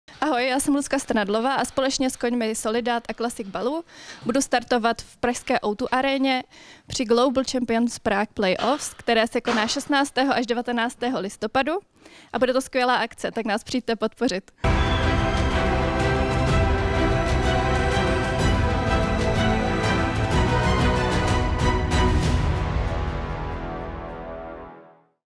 Bude to skvělá akce, přijďte nás podpořit, láká Lucie Strnadlová
Letos bude Lucie Strnadlová v pražské O2 areně startovat při soutěžích CSI2* s koňmi Solidat a Classic Balou a už se moc těší na svoji třetí účast při GC Playoffs. „Bude to skvělá akce, tak nás přijďte podpořit,“ vyzývá všechny parkurové fanoušky.